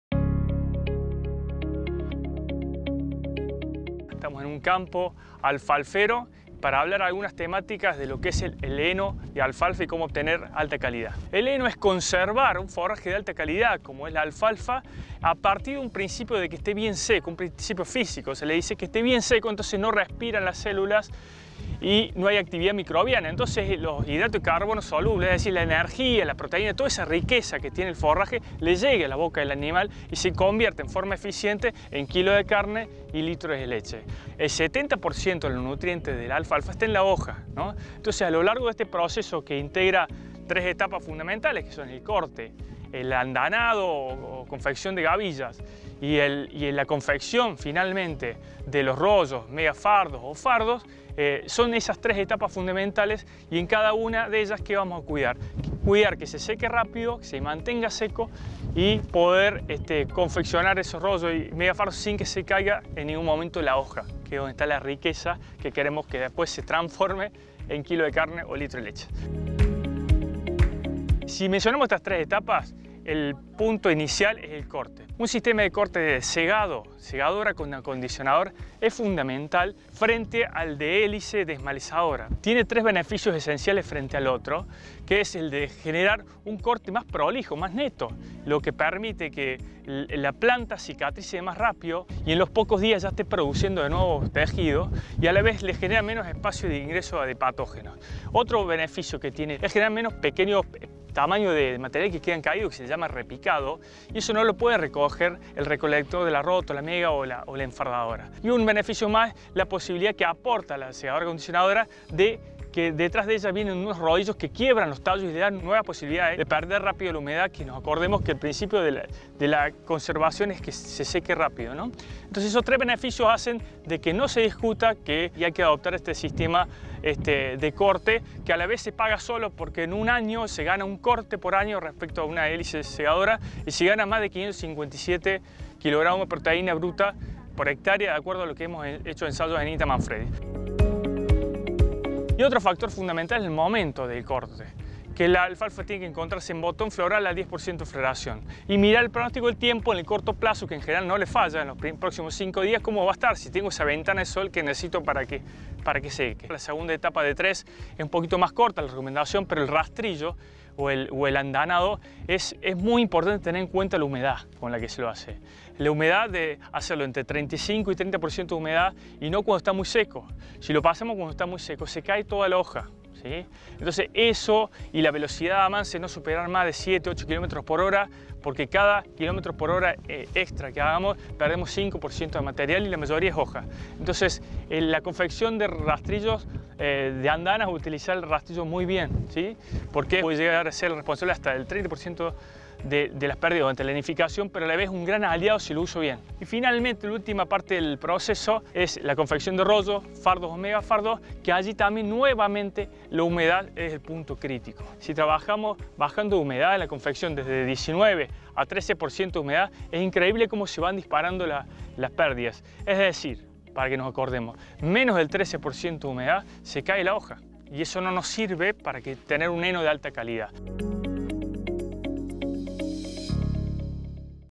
Producir heno de alfalfa de calidad
De ser una pradera extendida en 5 millones de hectáreas, la alfalfa ha reducido su superficie por la competencia de la agricultura. En este sentido, aumentar la eficiencia por hectárea esclave para que el cultivo rinda al máximo. Durante el henificado, esto supone cuidar el corte, el rastrillo y la confección para conservar los nutrientes.